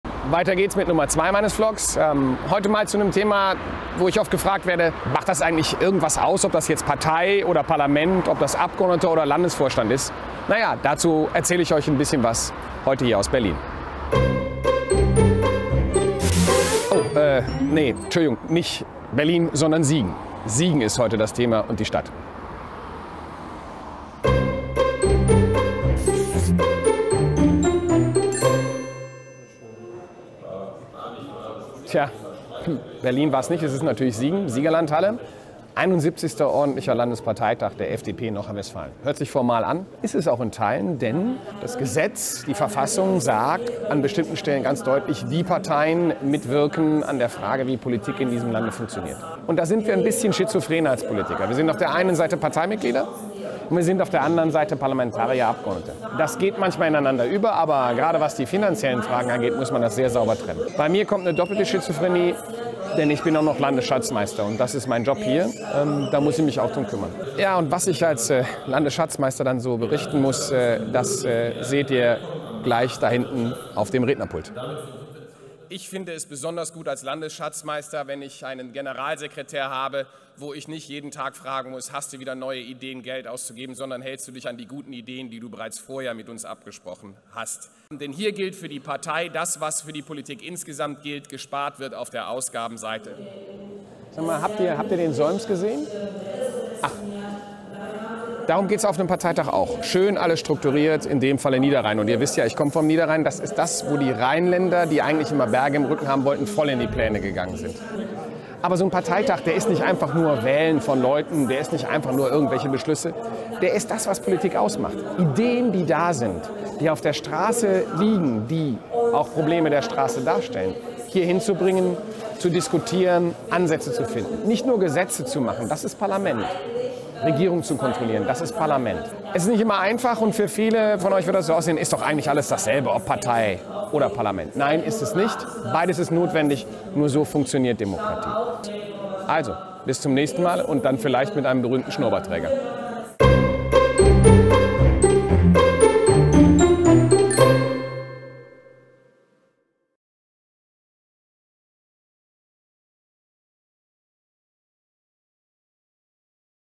Fraktion oder Partei? | Otto Fricke in Berlin 11011
Was ist Fraktion? Was ist Partei? Gibt es da überhaupt Unterschiede und wenn ja, welche? All das und ein kleiner Rückblick vom Landesparteitag in NRW gibt es dieses Mal bei Berlin 11011.